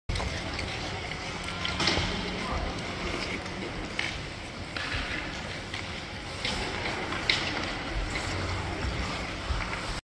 Jasmine Tran korspiruett Halmstad konståkning Mars 2023
Söndag 26 mars 2023

Lek o träning på allmänåkning. Sista dag sedan är ishallen stängt för säsongen.

 Jasmine 7 år .